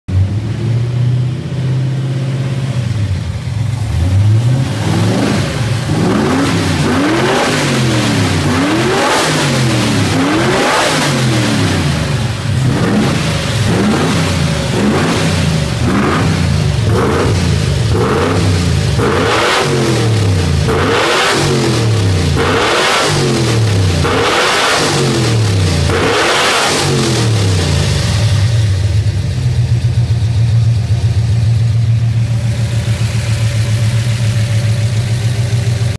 Ford Mustang 69 v8 289 con arbol headers balancines magnaflow distribuidor etc
asi suena ya con sus nuevos fierros.

carburador holley

araña edelbrock

arbol de levas

balancines

doble magnaflow

headers acero inoxidable

distribuidor msd

cables nology

entre otras cosas.